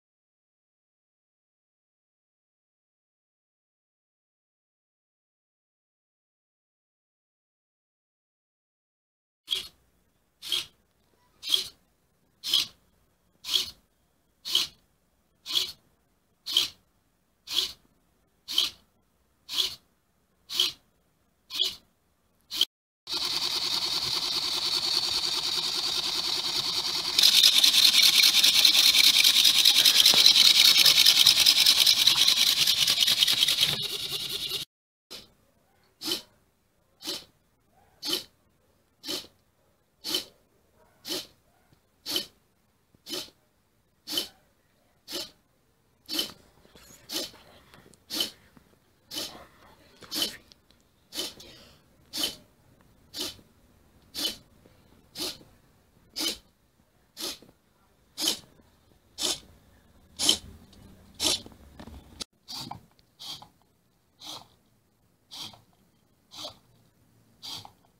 Robô de leoibing / Leonardo Ibing
Primeiro "robô" que eu criei no curso de mecatrônica e segundo na vida.
Segundo vídeo que eu, leoibing, fiz.
Só postei esse vídeo no Youtube para meu nome aparecer no Google quando pesquisado.